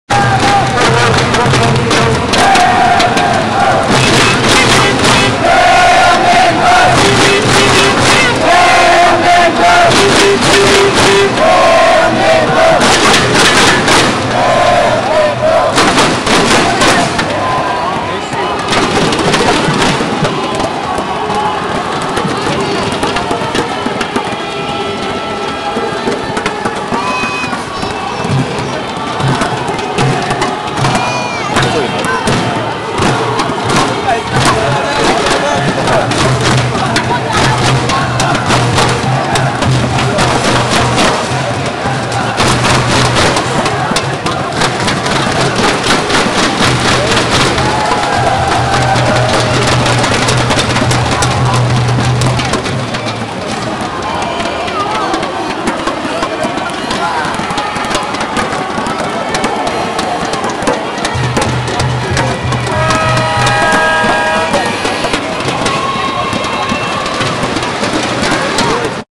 2010 South Africa Worldcup: KOREA Jeonju Worldcup Studium 전주월드컵 경기장 :
2010 South Africa Worldcup:  KOREA  Jeonju Worldcup Studium 전주월드컵 경기장 :
 : 전주한옥마을 Jeonju Hanok Village.  :
Jeonju city in KOREA.
Jeonju city.（전주시. 全州市）. KOREA North Jeolla Province.
 Jeonju Hanok village and Jeonju Bibimbab((a mixed rice/veggie bowl-best food) is so famous in the world.